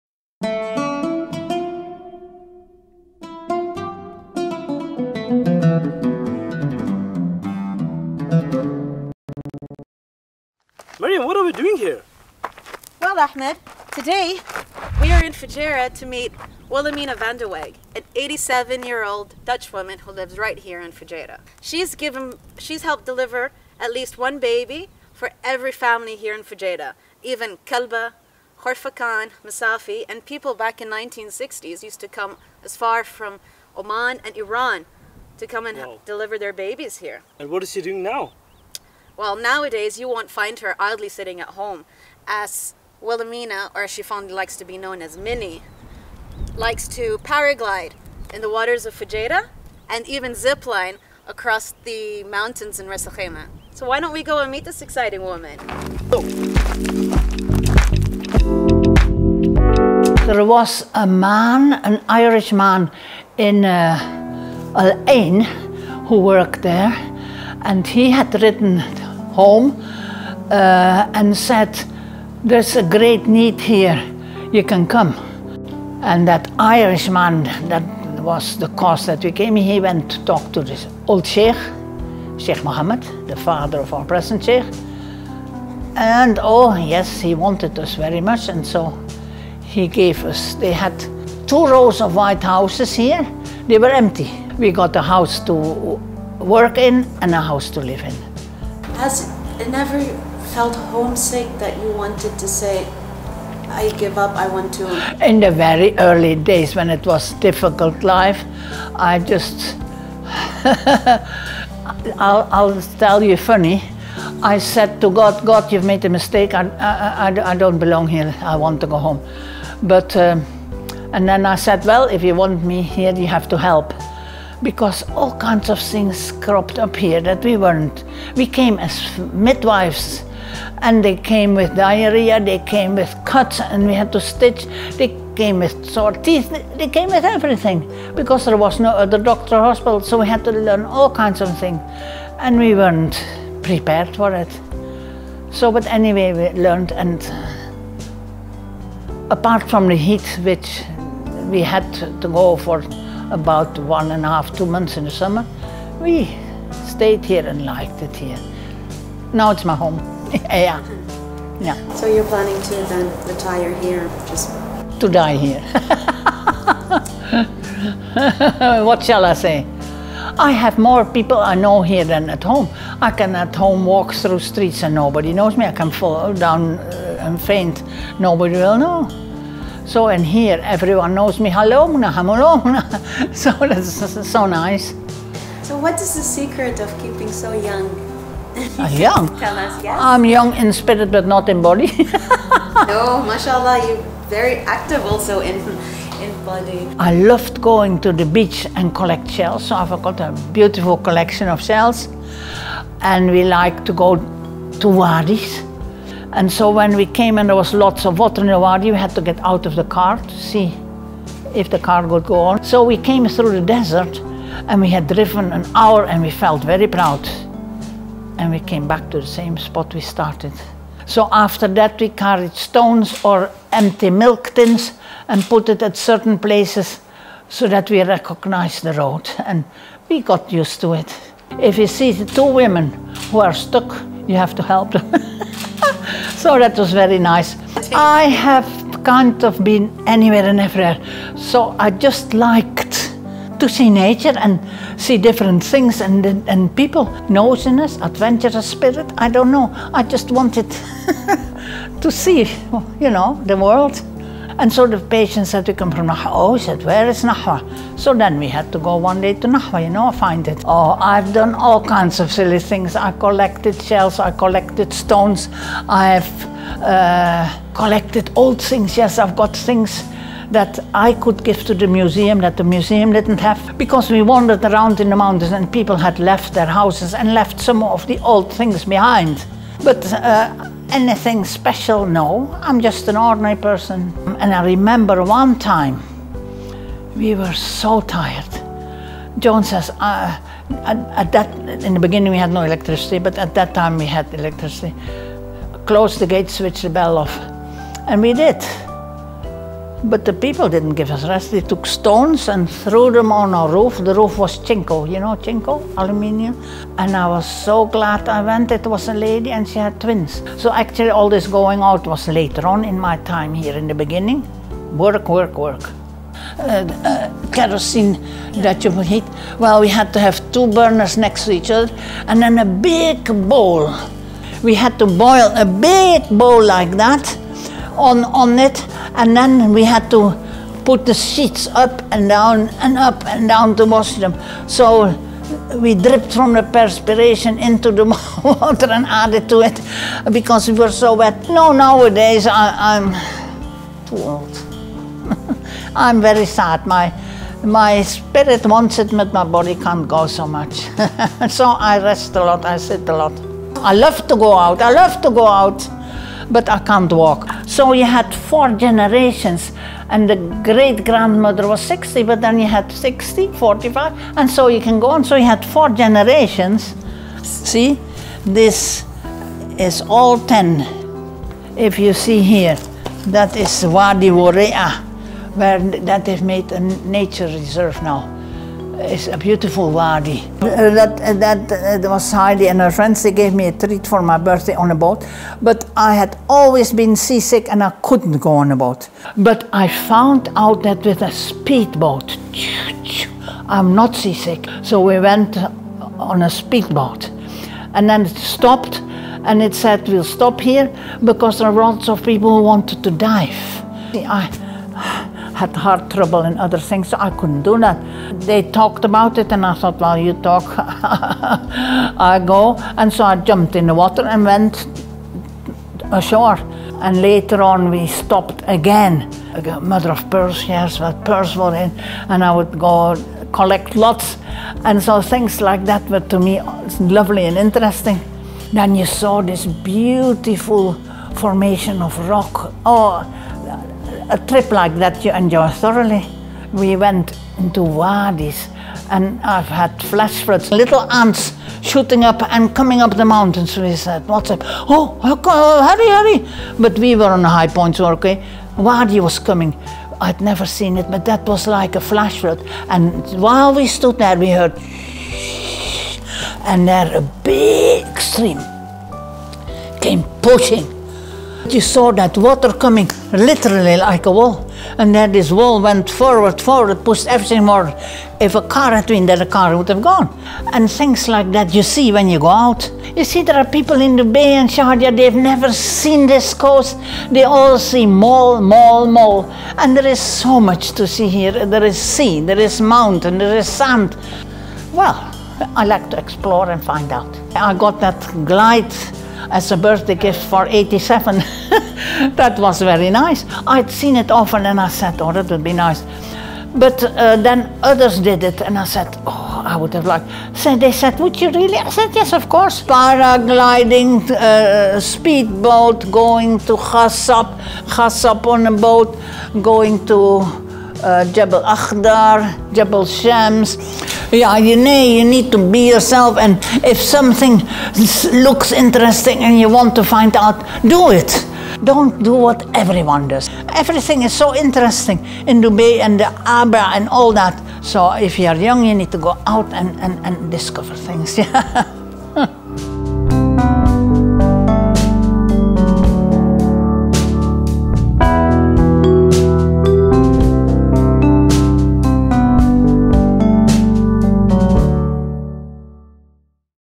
From Fujairah midwife to 87-year-old paraglider
The rugged mountains, wadis and desert sand of the UAE’s eastern coast is a far cry from Holland’s canals, windmills and tulip fields. But after living in the UAE for 55 years, Fujairah is where Wilhelmina van de Weg calls home. See the full story on gulfnews.com